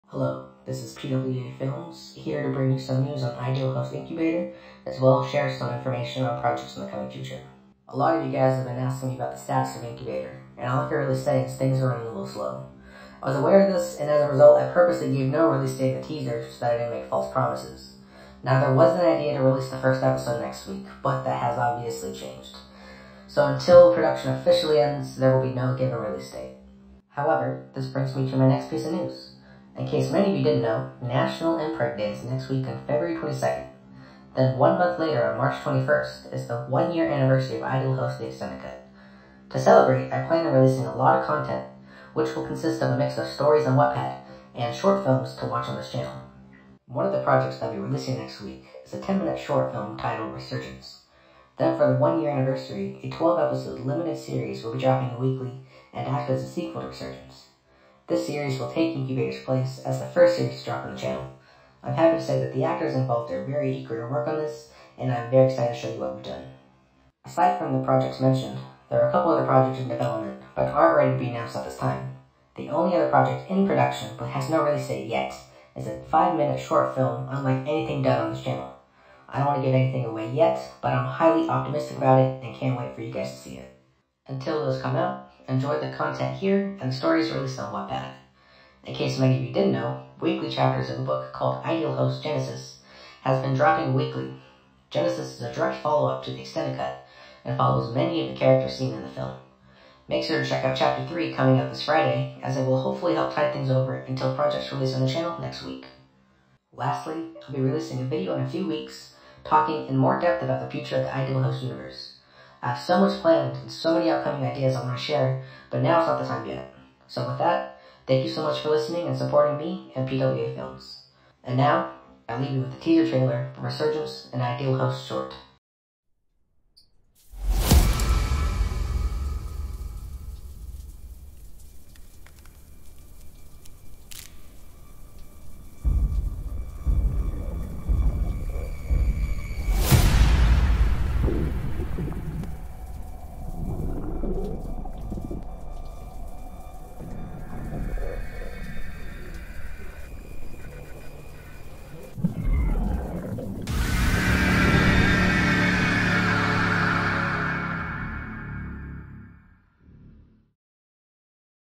Announcement 003 - What's Happening
Addressing Ideal Host: Incubator and talking about what is coming in the coming weeks! Sneak Peek at end.